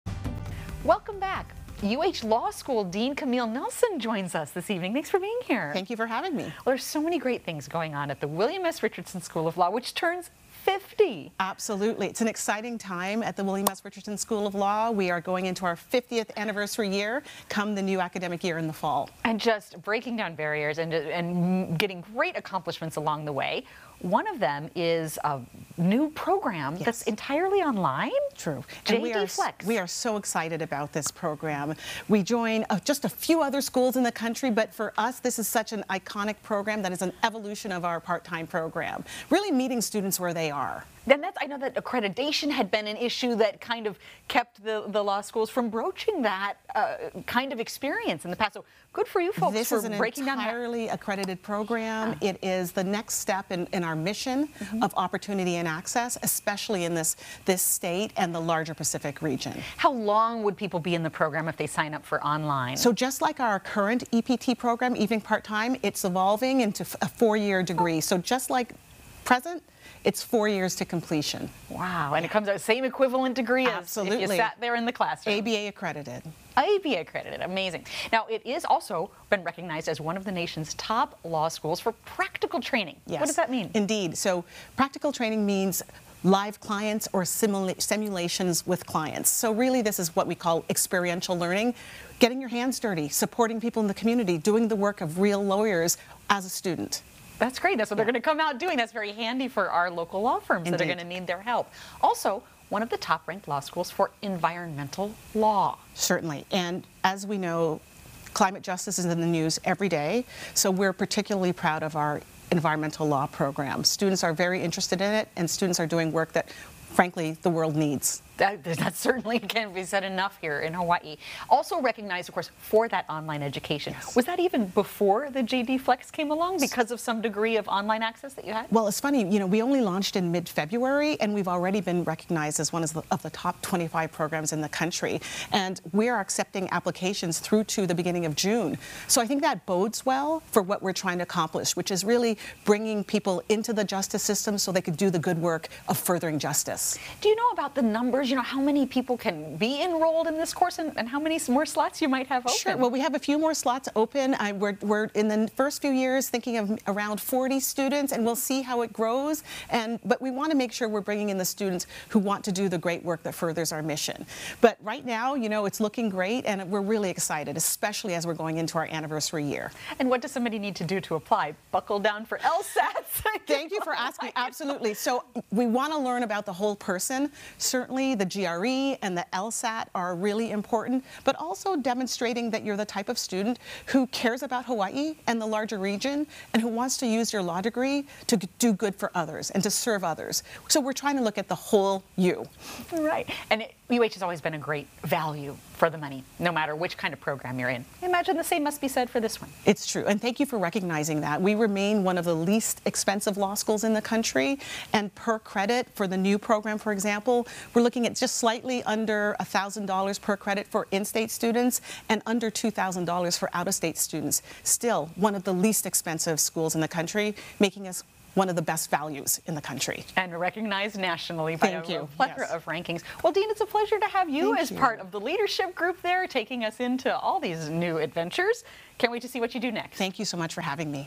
UH law school announces new online, part-time JD flex program
University of Hawaii at Manoa William S. Richardson School of Law has launched a new part-time, online program for students who want to earn a law degree.